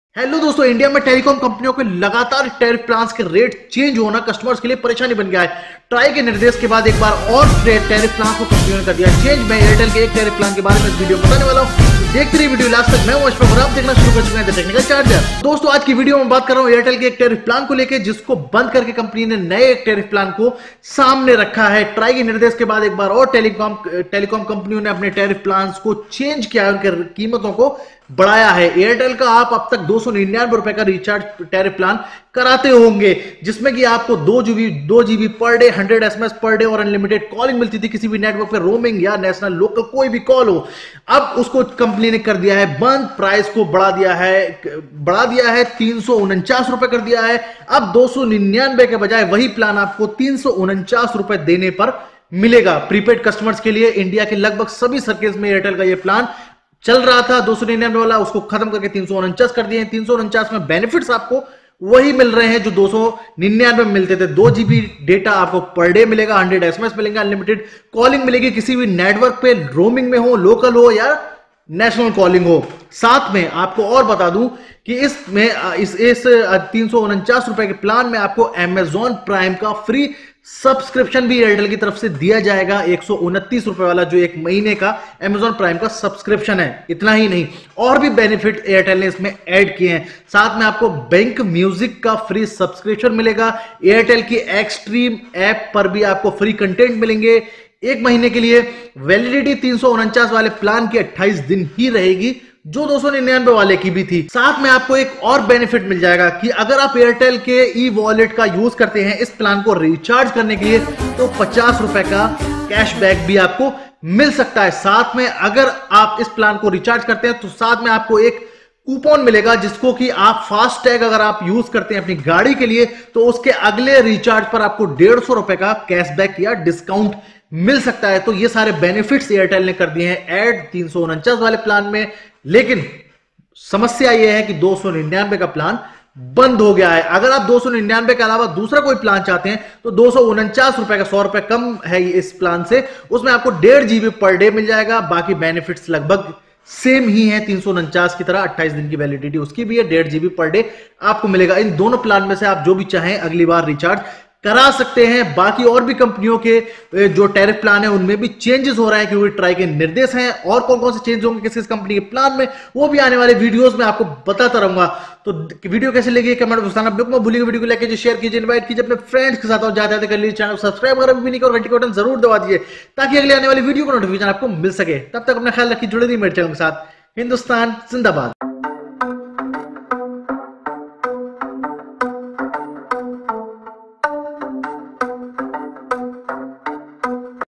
Airtel 299 Prepaid plan changed into 399 Tariff Plan
Hello Doston,
Airtel ne 299 wala plan band kar diya hai aaj se isko 349 me change kar diya hai,
Thanks